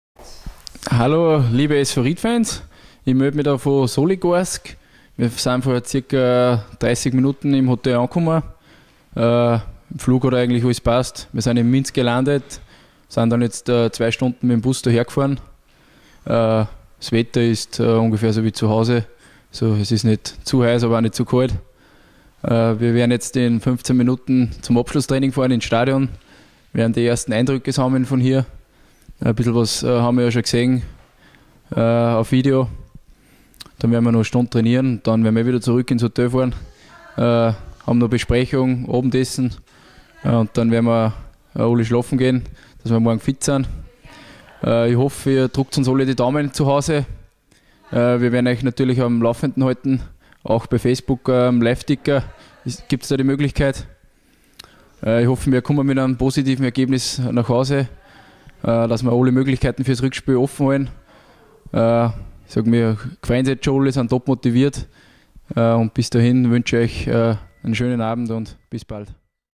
Thomas Reifeltshammer mit den ersten Eindrücken aus Weißrussland
Thomas Reifeltshammer weiht den SVR-Youtube-Channel ein und meldet sich erstmals aus Soligorsk. Der Abwehrchef schildert seine ersten Eindrücke aus Weißrussland und den weiteren Tagesablauf in Soligorsk.